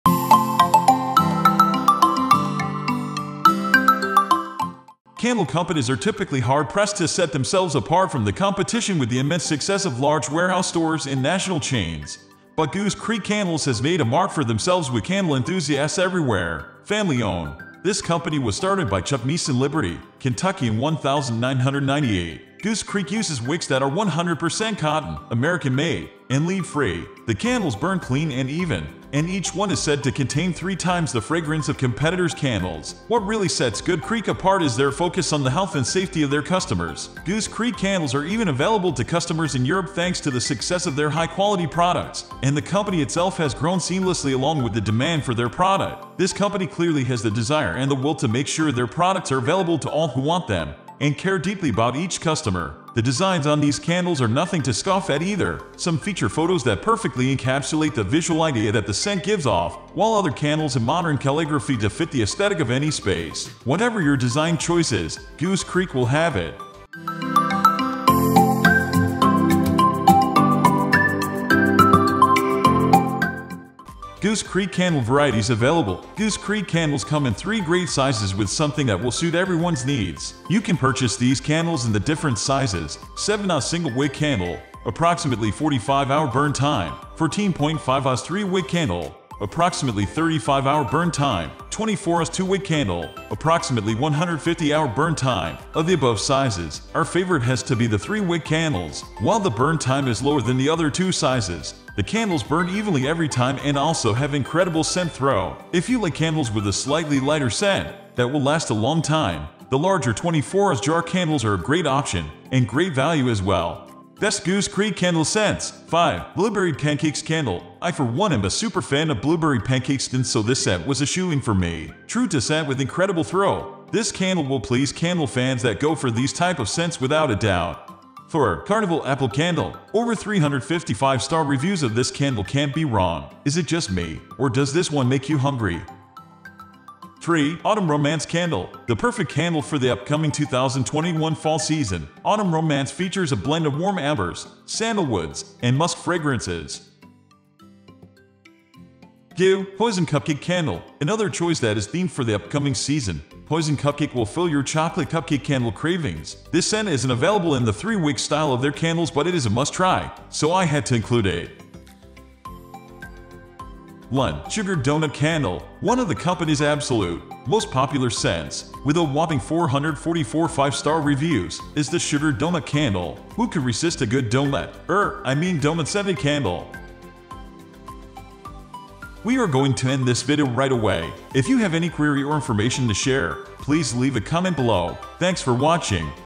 Goose Creek Candles Review: Is It Worth The Money?
Candle companies are typically hard-pressed to set themselves apart from the competition with the immense success of large warehouse stores and national chains, but Goose Creek Candles has made a mark for themselves with candle enthusiasts everywhere.